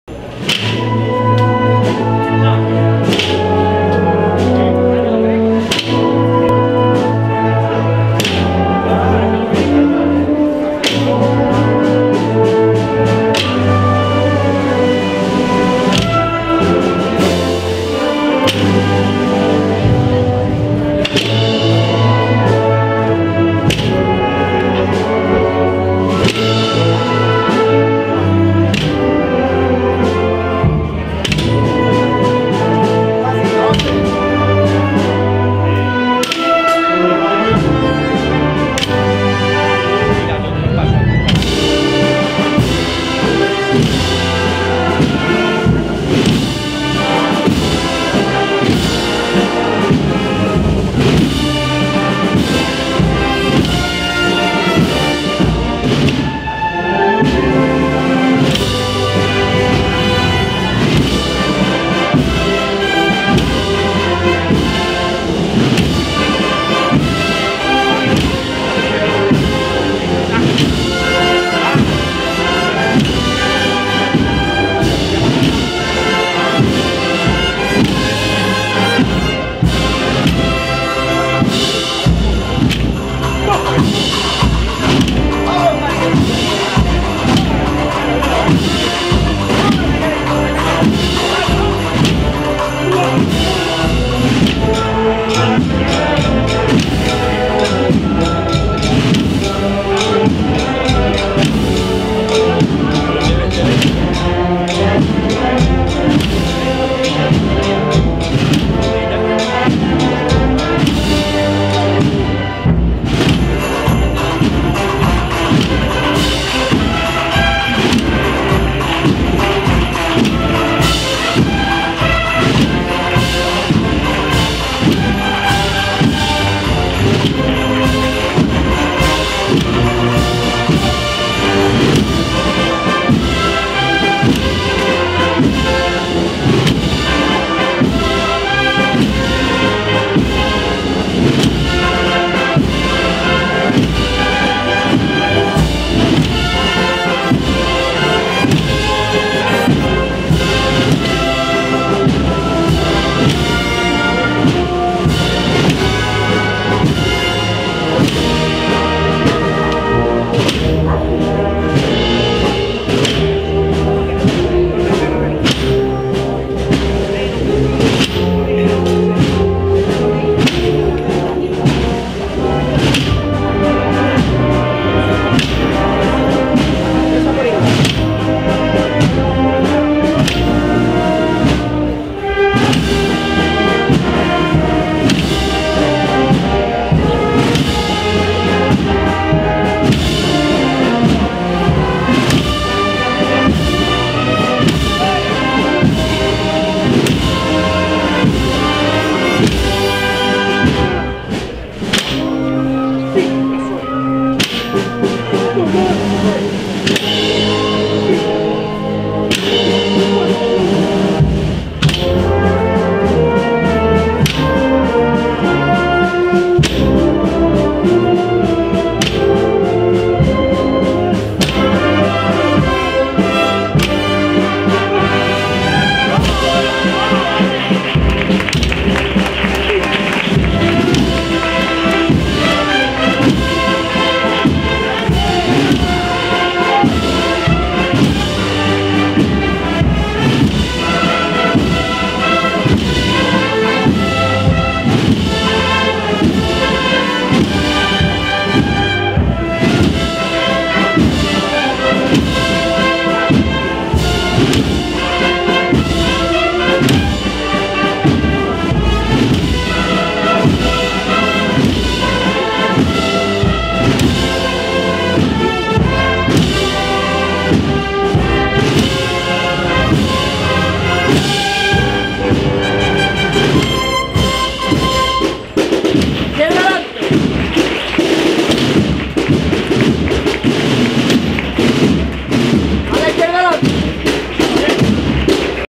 Ntra. Sra. del Rosario Coronada"Traslado de Vuelta a Santo Domingo"(Banda de Música Maestro Dueñas)
(Cadiz)  23/6/2019